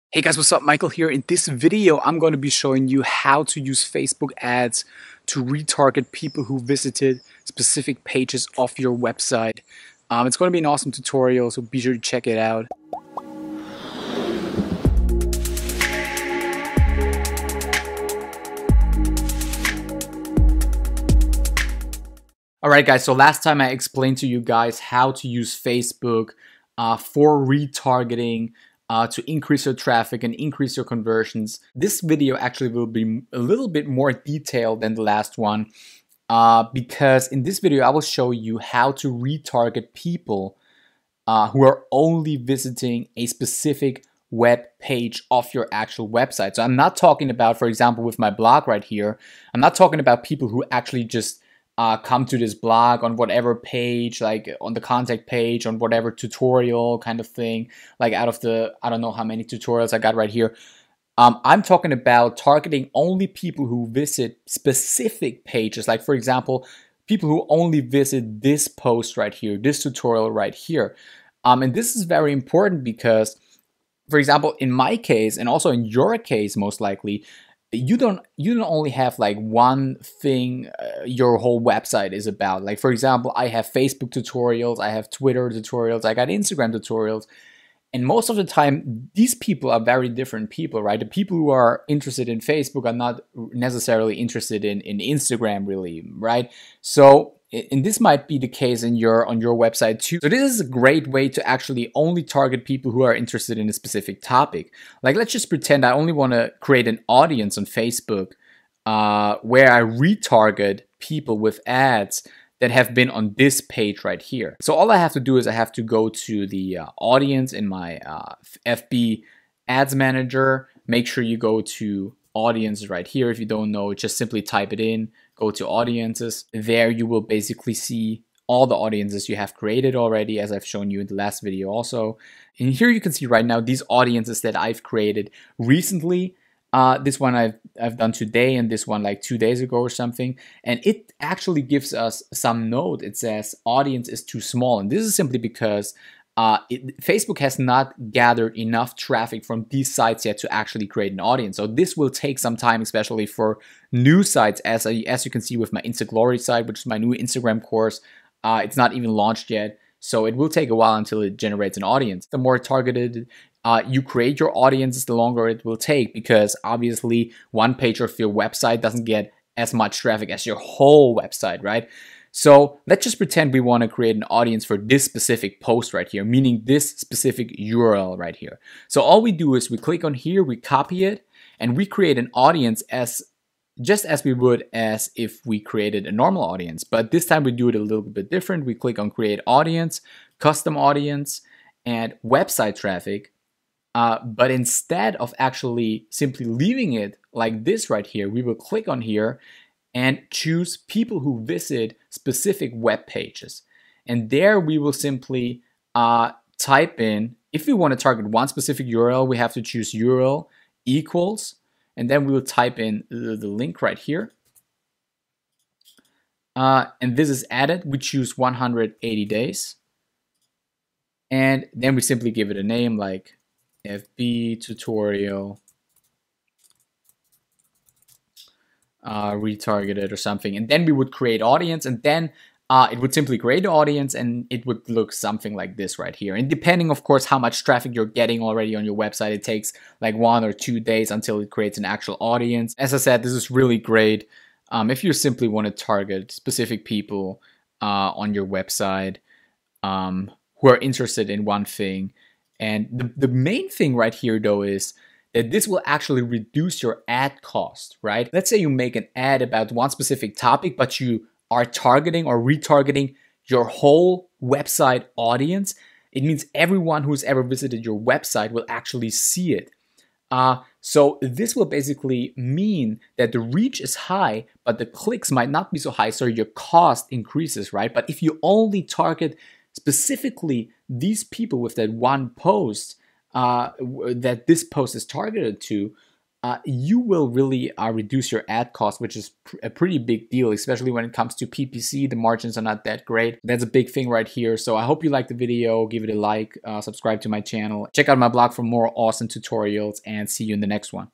How To Create Custom Facebook Audiences - Facebook Retargeting
In this video I will be showing you How To Create Custom Facebook Audiences by targeting only people who visited specific webpages of your site.

I hope you liked the video and if you have any questions please let me know!